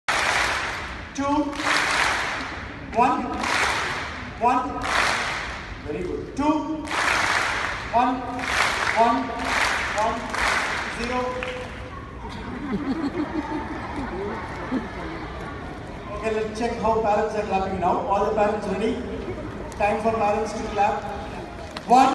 brainobrain delhi nation fest 2018 prt 4
brainobrain delhi nation fest 2018 held in delhi. In mass level, children has come to delhi and perform their mind alertness act.